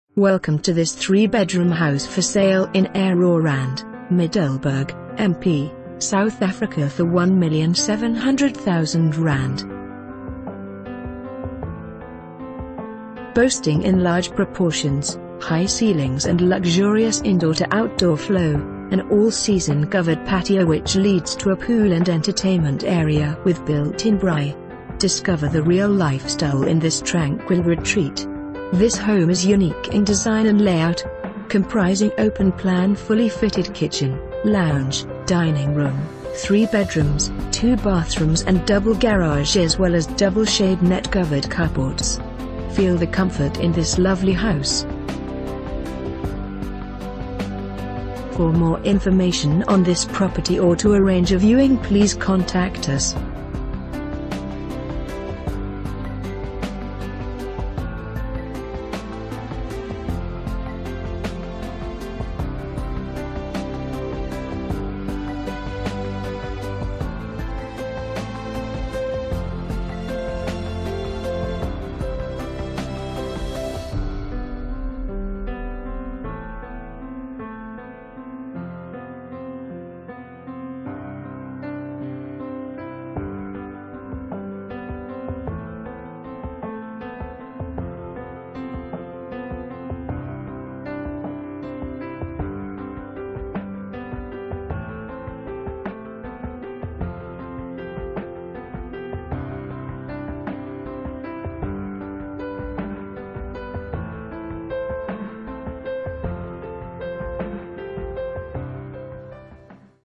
3 Bedroom House For Sale in Aerorand, Middelburg - MP, South Africa for ZAR 1,700,000...
Boasting in large proportions, high ceilings and luxurious indoor to outdoor flow, an all season covered patio which leads to a pool and entertainment area with built-in braai.  Discover the real lifestyle in this tranquil retreat.  This home is unique in design and layout.  Comprising open plan fully fitted kitchen, lounge, ...